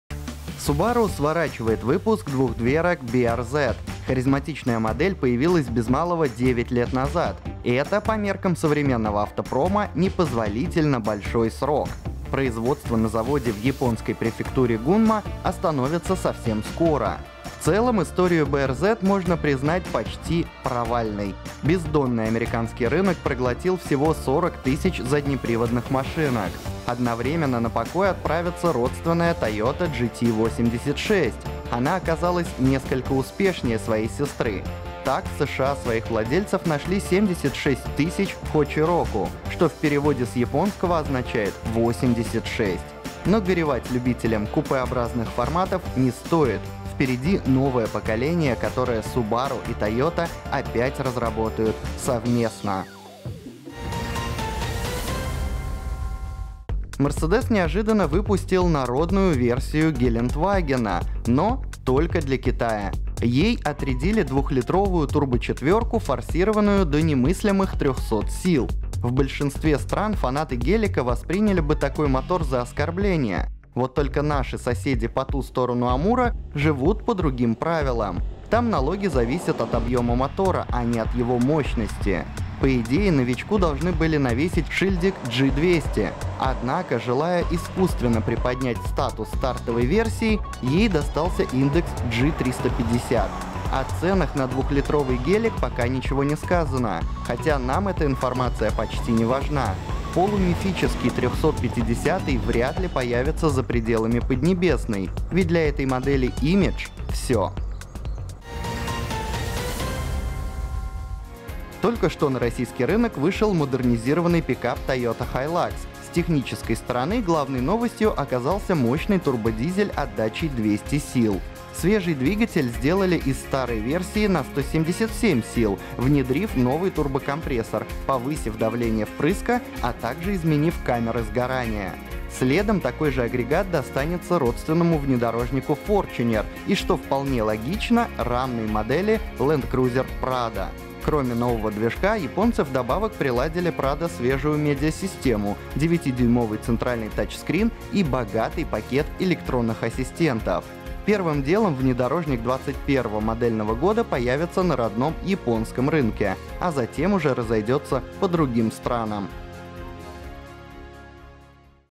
Subaru BRZ и Toyota GT86 на пенсии. 2 литра для Гелика. Обновление TLC200 | Новости с колёс №1042
Subaru сворачивает выпуск двухдверок BRZ. Харизматичная модель появилась без малого девять лет назад. Это, по меркам современного автопрома, непозволительно большой срок. Производство на заводе в японской префектуре Гунма остановится совсем скоро. В целом историю BRZ можно признать почти провальной. Бездонный американский рынок проглотил всего 40 тысяч заднеприводных машинок. Одновременно на покой отправится родственная Toyota GT86. Она оказалась несколько успешнее своей сестры. Так, в США своих владельцев нашли 76 000 штук.

Mercedes-Benz неожиданно выпустил «народную» версию «Гелендвагена». Но — только для Китая. Ей отрядили двухлитровую «турбочетвёрку», форсированную до немыслимых трёхсот сил. В большинстве стран фанаты «гелика» восприняли бы такой мотор за оскорбление. Вот только наши соседи по ту сторону Амура живут по другим правилам: там налоги зависят от объёма мотора, а не от его мощности.

Только что на российский рынок вышел модернизированный пикап Toyota Hilux. С технической стороны главной новостью оказался мощный турбодизель отдачей 200 сил. Свежий двигатель сделали из старой версии на 177 сил, внедрив новый турбокомпрессор, повысив давление впрыска, а также изменив камеры сгорания. Следом такой же агрегат достанется родственному внедорожнику Fortuner и, что вполне логично, рамной модели Land Cruiser Prado.